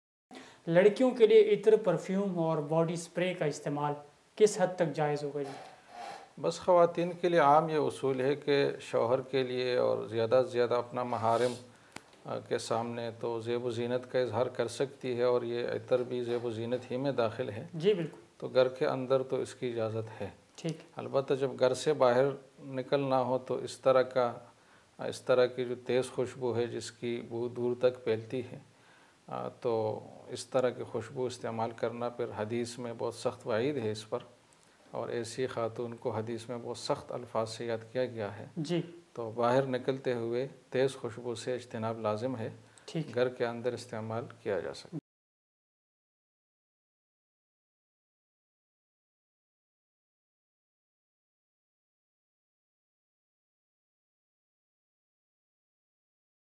Khawatin Ke Liye Etter Ke Istemal Ka Hukm ? | Jamia-Tur-Rasheed
Khawatin Ke Liye Etter Ke Istemal Ka Hukm  ?
  خواتین کےلیے عطرکےاستعمال کاحکم؟
Mufti Muhammad Husain KHalil Khail    Sb

مفتی محمد حسین خلیل خیل صاحب

استاذ جامعۃالرشیدکراچی

Category
Education